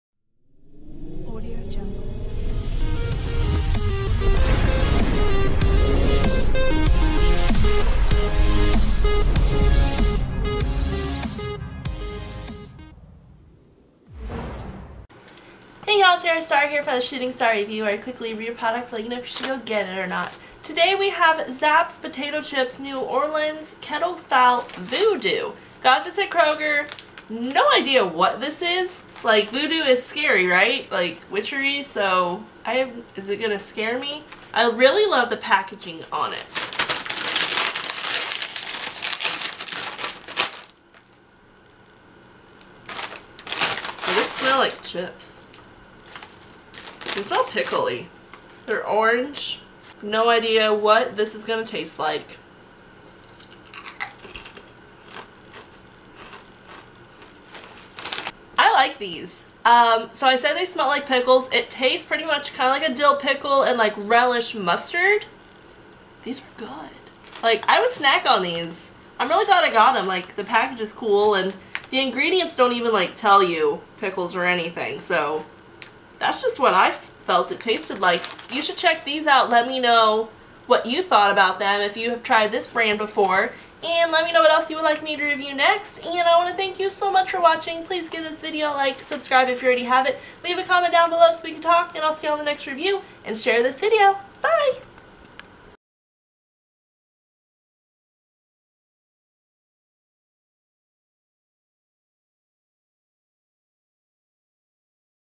SSR: Zapp's® Voodoo™ Potato Chips
Hey Y'all!! Today I'm having a taste New Orleans style with Zapp's® Voodoo™ Potato Chips. Got them at Kroger and decided hey why not try these out. Let me know if you've ever had these and what you think.